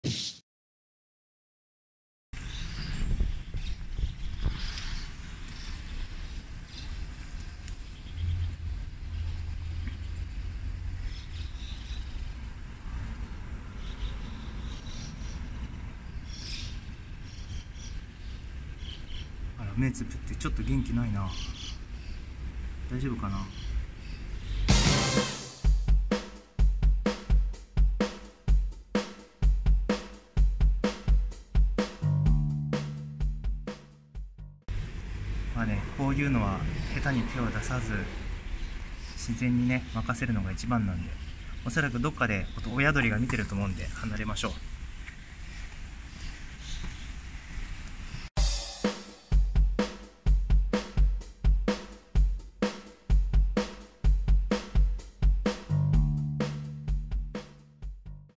あなたなら保護する？道端に落ちている鳥のヒナを発見しました！
僕なら例え外傷があってもむやみに保護をしないです。
（生態系保全の意味で保護するべき動物ならケースバイケースですが）
ちゃんと他の野生動物の餌になるからです。